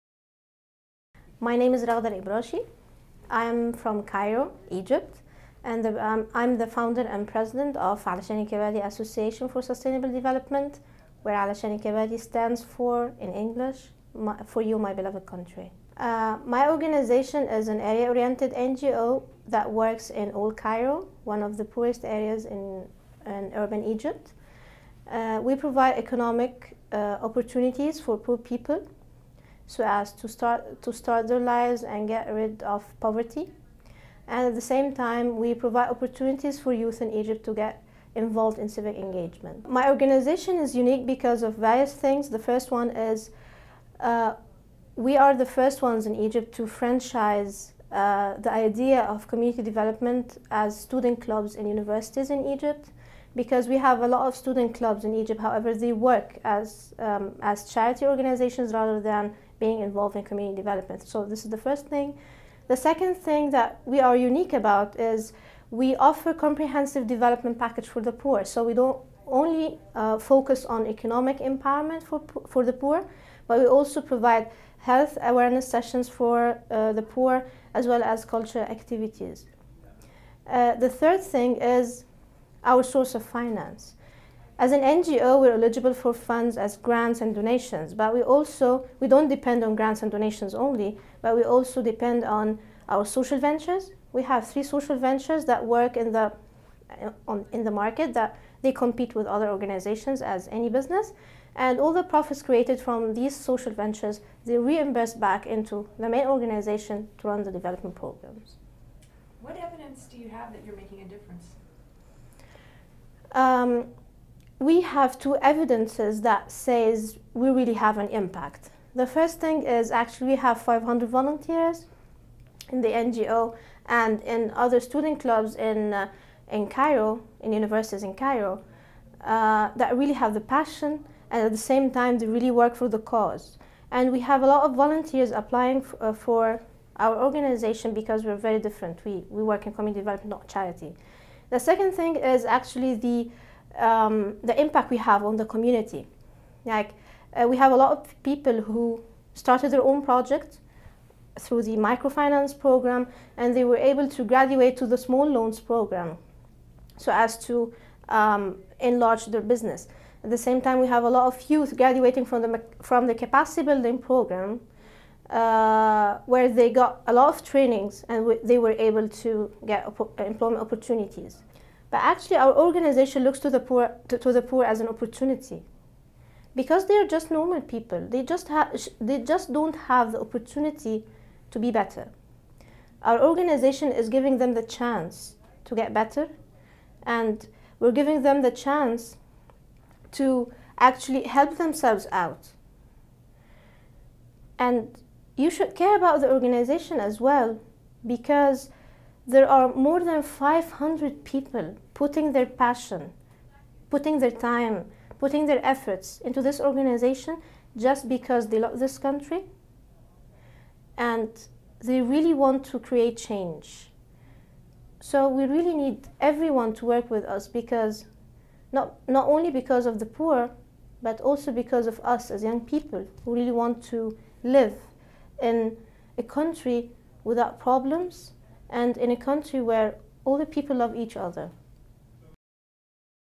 Raghda Mohamed Mamdouh El Ebrashi - 2008 YouthActionNet Fellows
Raghda is the Founder and Chairperson of the Alashanek ya Balady Association for Sustainable Development (AYB-SD). Alashanek ya Balady translates from Arabic to mean "For you, my beloved country." The organization works on the development of poor urban areas in Egypt through various economic, social, and cultural projects.